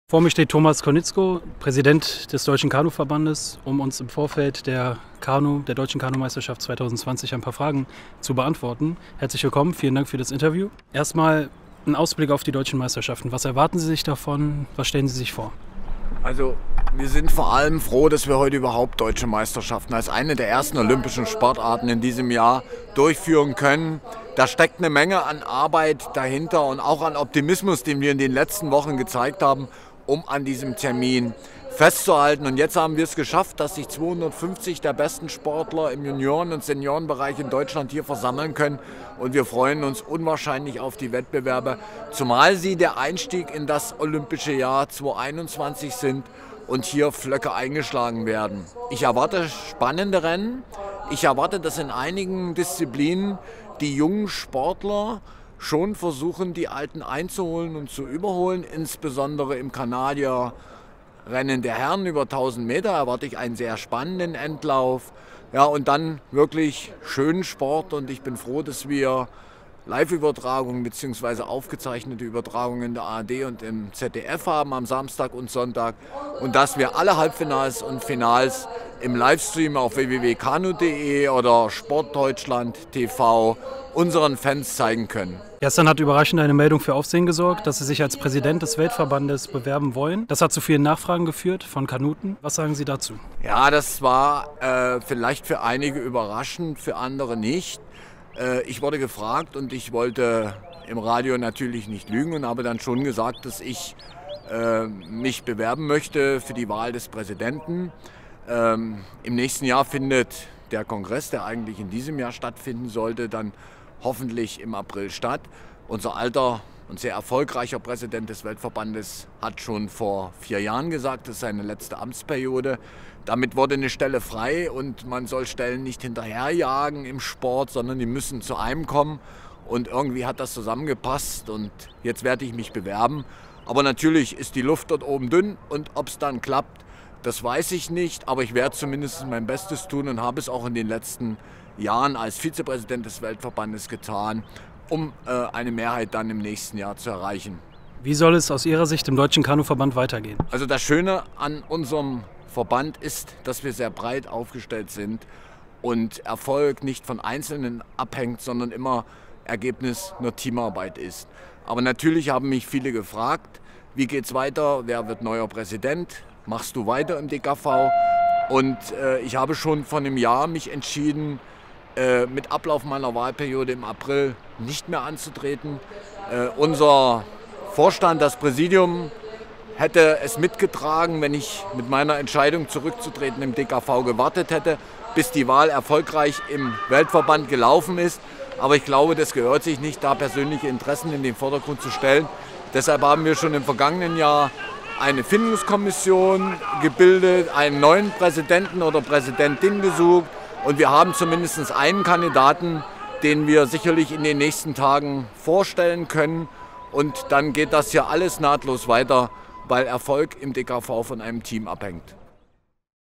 Interview mit Thomas Konietzko
Eine Entscheidung die für Aufsehen sorgte: Thomas Konietzko kandidiert für das Amt des Weltverband-Präsidenten. Was er verändern möchte und wie seine Strategie ist, erfahrt Ihr im Interview.